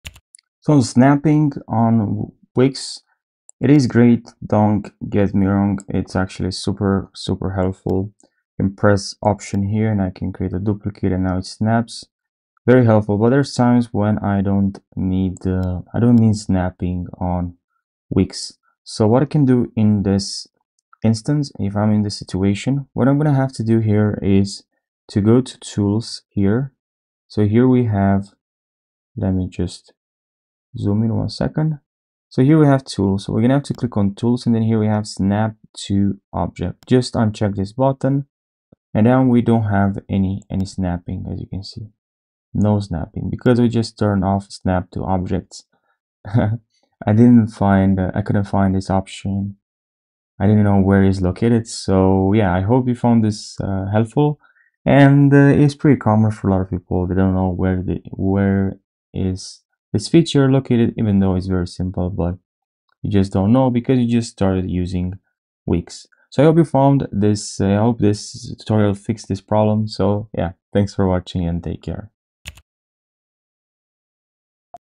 How Can I Turn Off Snapping On Wix Website Builder Turn Off Snapping
In this wix tutorial I'm gonna show you how can i turn off snapping on wix website builder turn off snapping